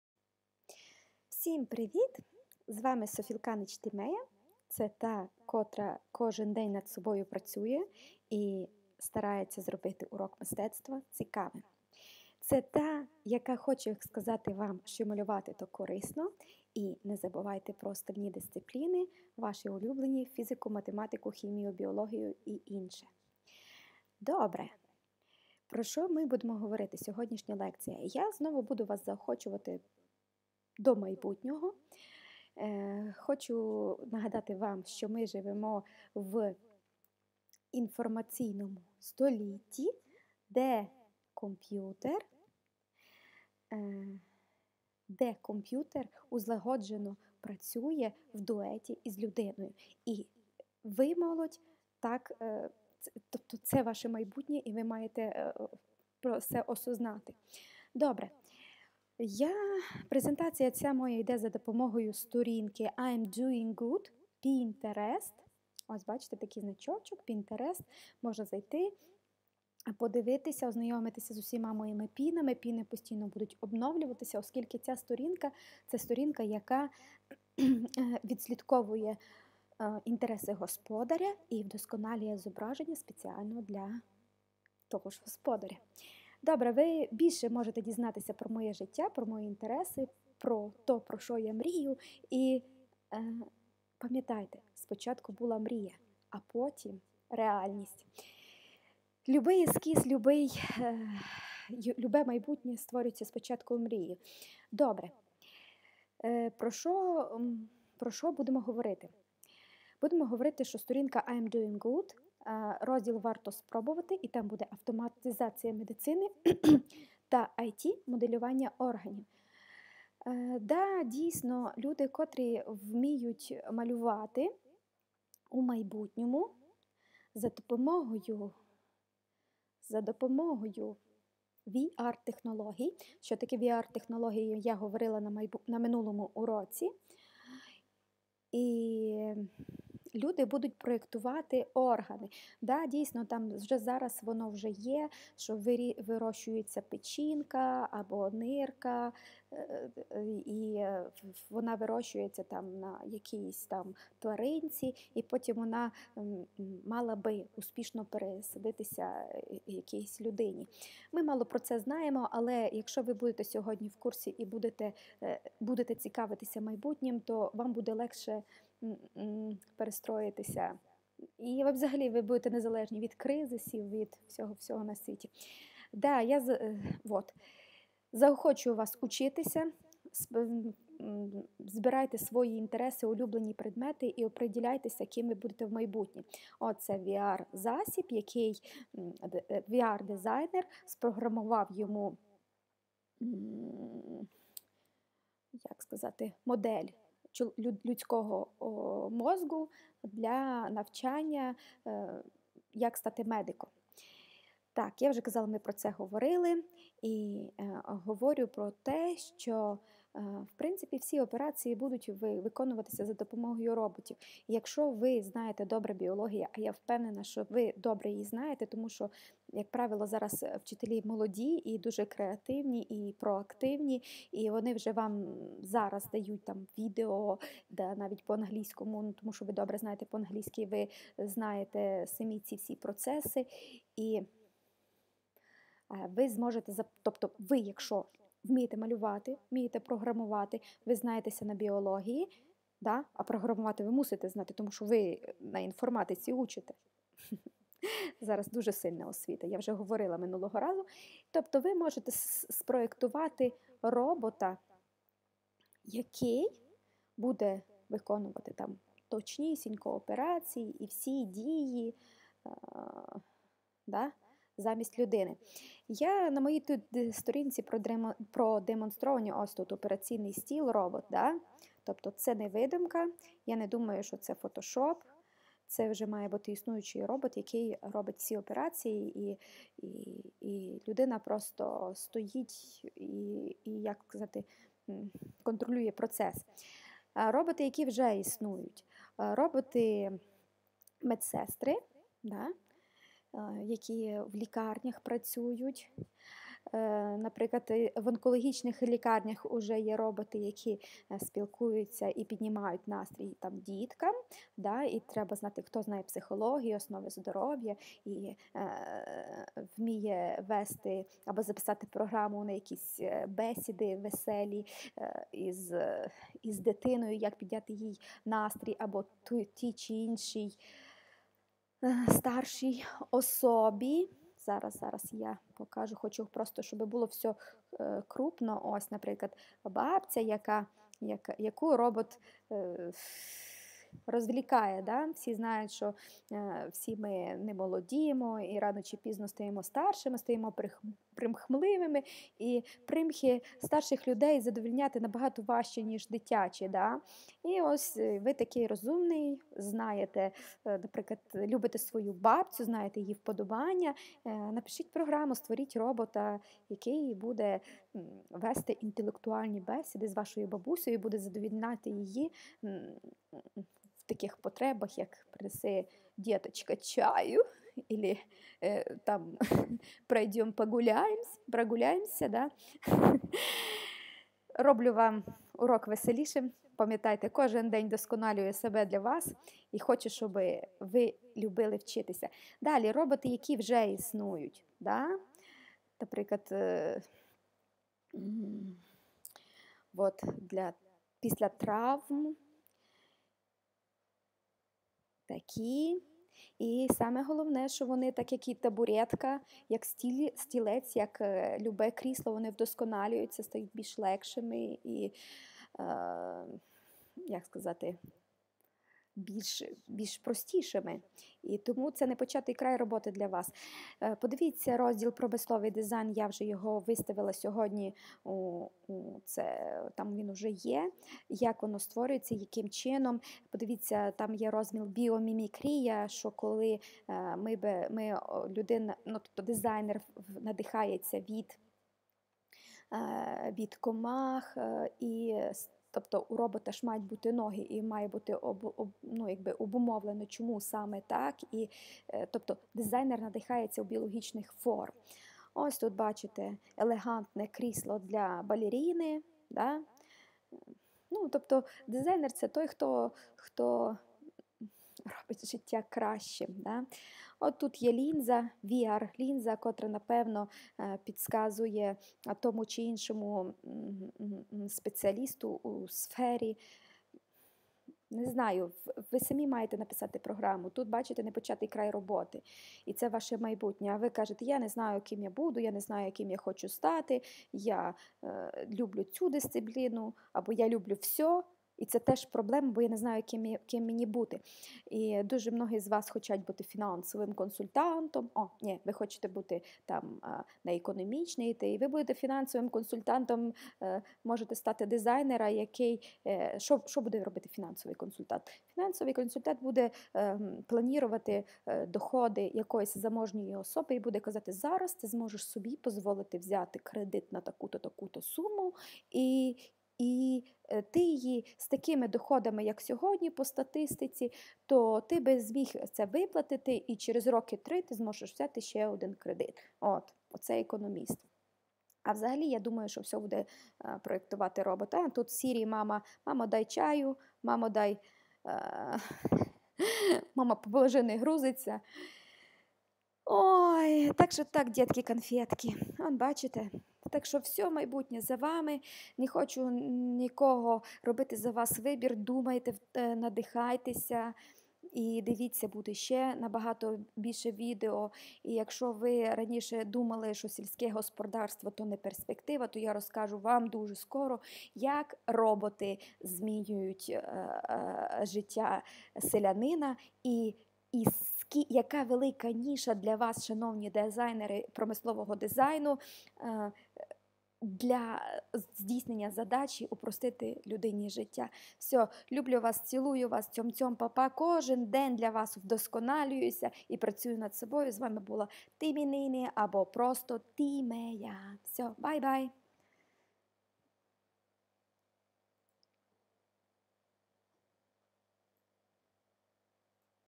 Професії майбутнього. Автоматизація медицини або ІТ дизайн (моделювання) органів людського тіла.
Календарне планування Мистецтво 9 клас «Яким я бачу ХХ століття». "Професії майбутнього №1, №2 та №3"
Презентація сторінки Pinterest    від Timea Sofilkanych  Професії майбутнього. Автоматизація медицини або ІТ дизайн (моделювання) органів людського тіла.